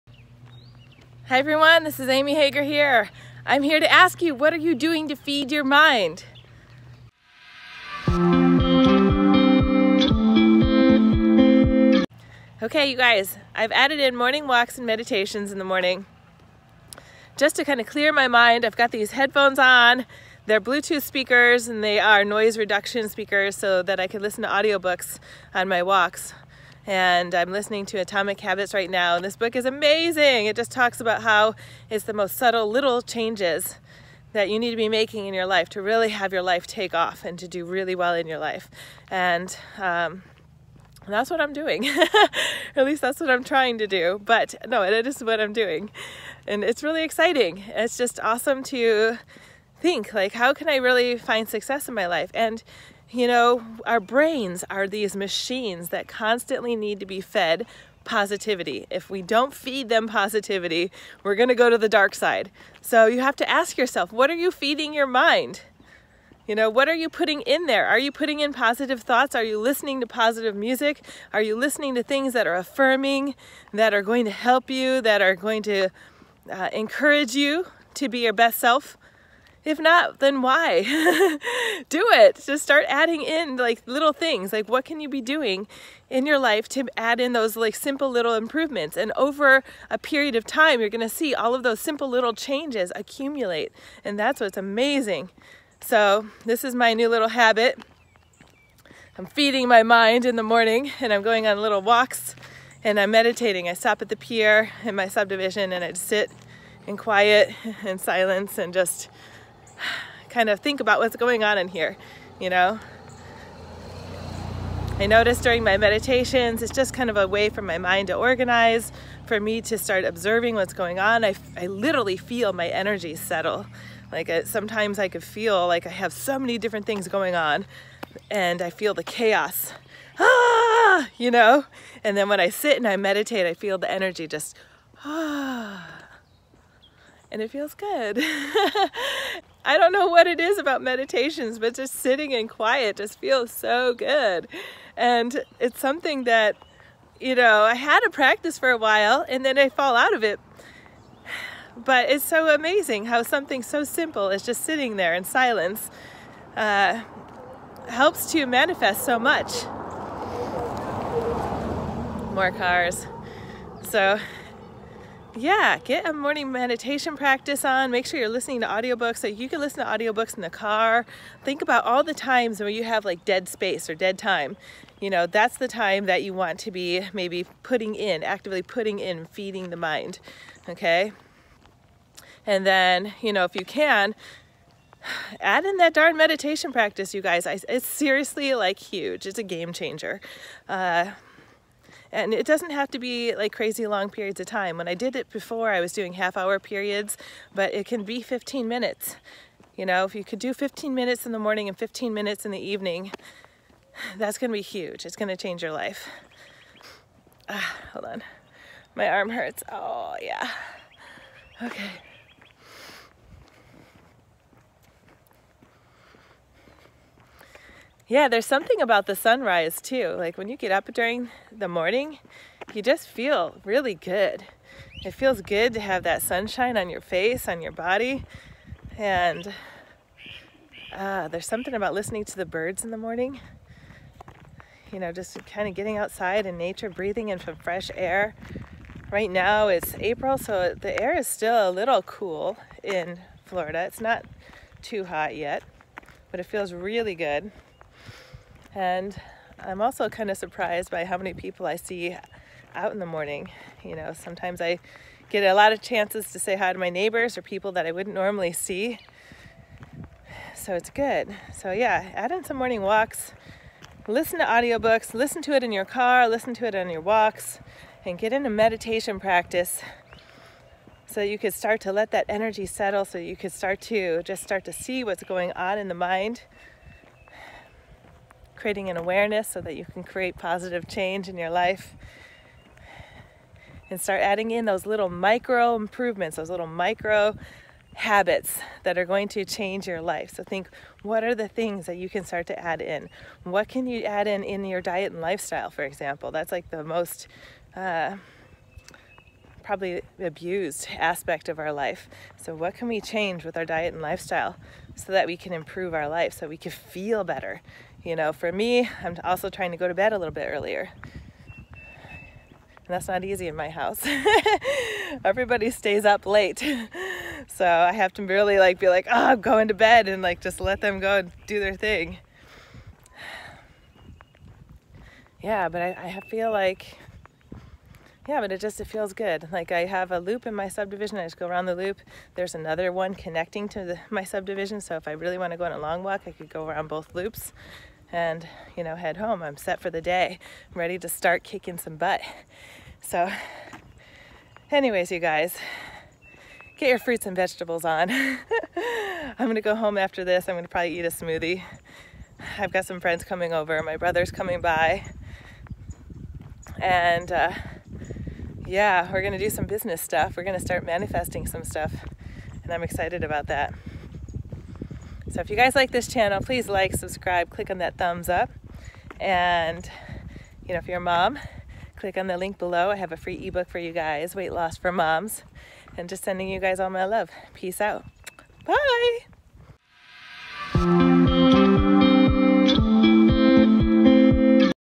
What Are You Feeding Your Mind? What Micro Improvements in Your Habits Can You Make?
Hello Everyone,

This video is about what are you feeding your mind?

What are you doing to create a healthy positive thoughts?

Do you have a meditation practice?

What Micro Improvements in Your Habits Can You Make?

I help Moms find their Ideal Body Weight without dieting using a whole foods plant based lifestyle.

Much Love!

Amy